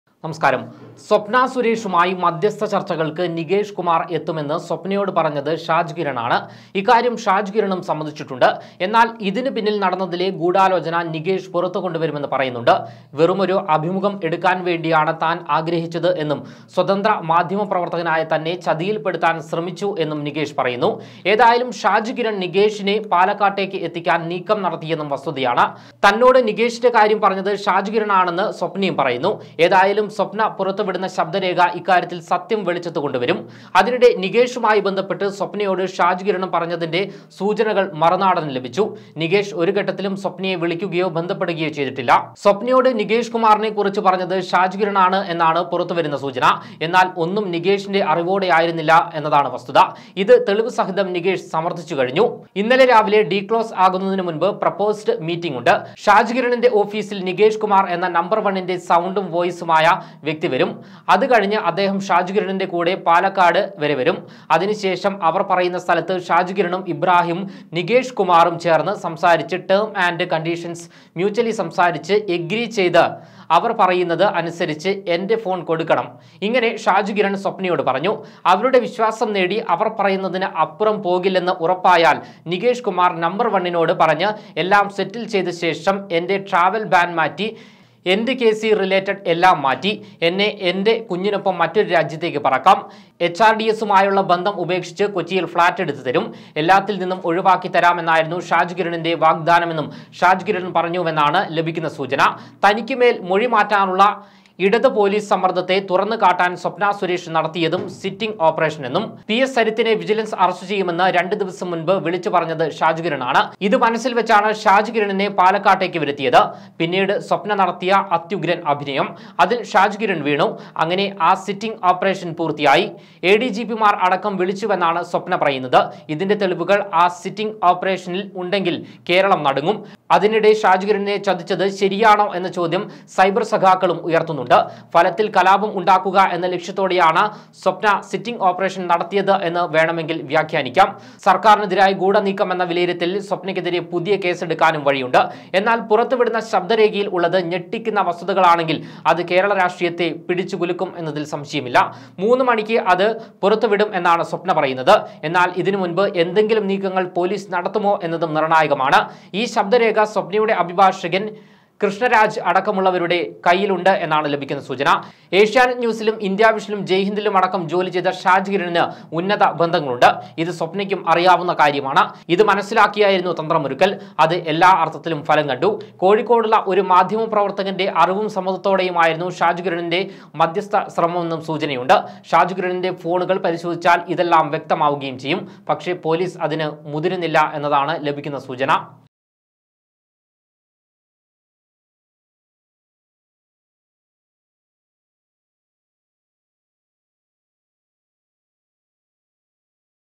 നടന്നത് നികേഷ് അറിയാതെയുള്ള ഗൂഢാലോചന... | shaj kiran
പ്രൊപ്പോസ്ഡ് മീറ്റിംഗിൽ നികേഷ് എത്തും; അതിന് ശേഷം പാലക്കാട്ടേക്ക് ഞങ്ങൾ വരും; പറയുന്നിടത്ത് വന്ന് ചർച്ചകളിലൂടെ ധാരണയുണ്ടാക്കാം; ആ ഫോൺ നികേഷിന് കൊടുക്കണം; അതു കഴിഞ്ഞാൽ കുഞ്ഞുമൊത്ത് വിദേശത്തേക്ക് പറക്കാം; കൊച്ചിയിൽ ഫ്‌ളാറ്റും കിട്ടും; 'ഒന്നാം നമ്പറിൽ' ഷാജ് കിരൺ പറഞ്ഞത് ഇങ്ങനെയോ? നടന്നത് നികേഷ് അറിയാതെയുള്ള ഗൂഢാലോചനയും